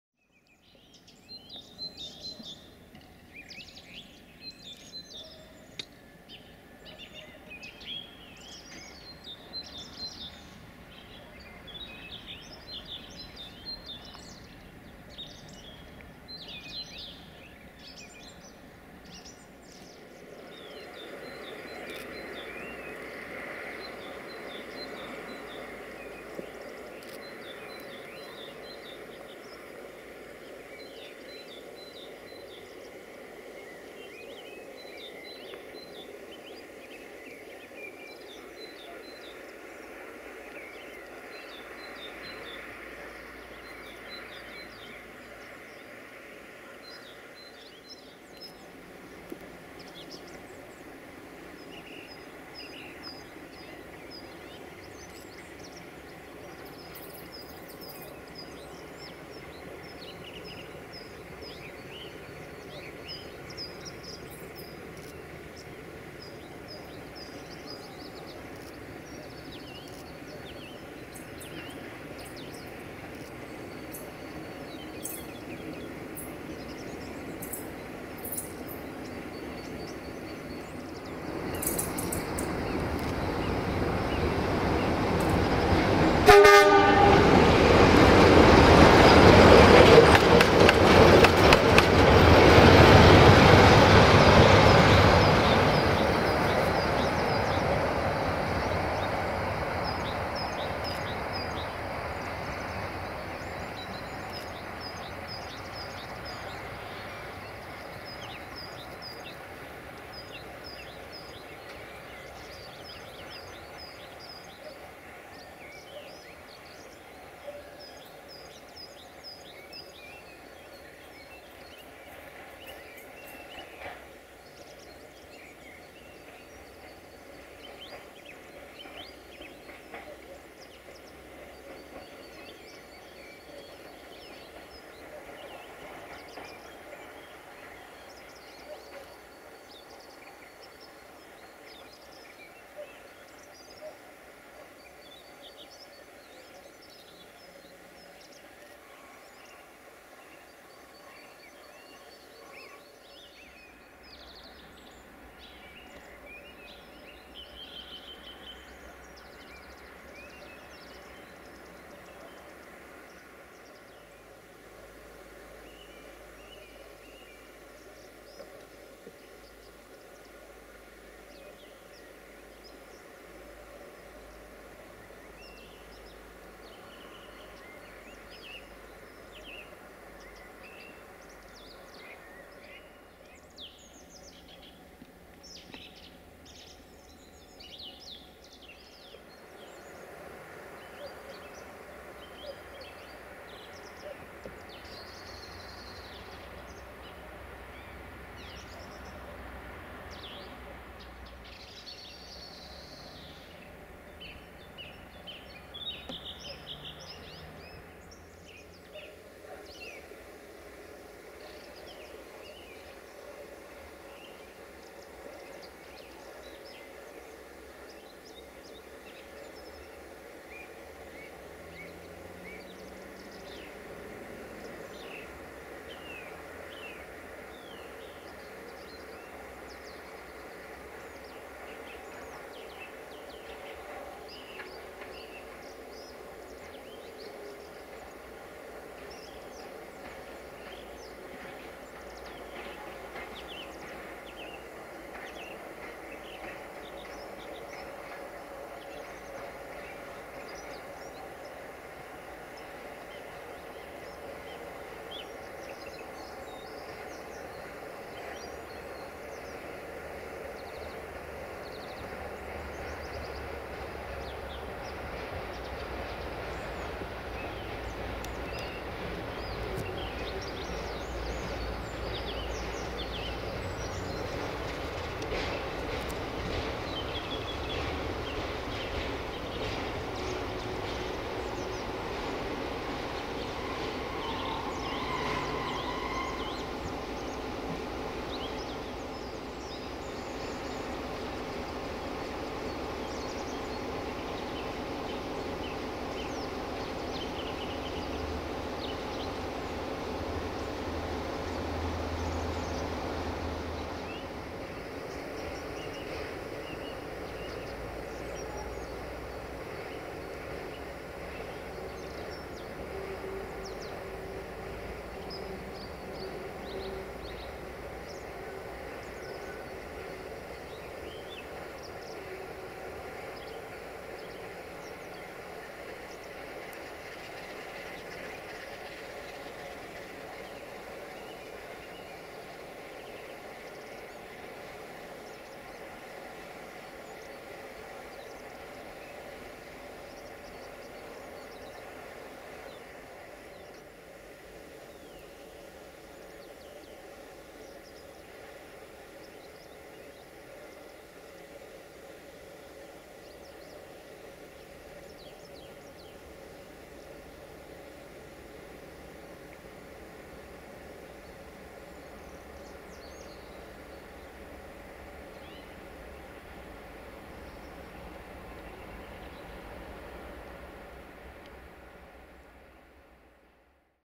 LDE2100 60-0041-8 RO-UTZ in Gara Jibou Station pe Seara/on Evening - 04 March 2017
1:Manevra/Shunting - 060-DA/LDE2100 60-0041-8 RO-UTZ

04 March 2017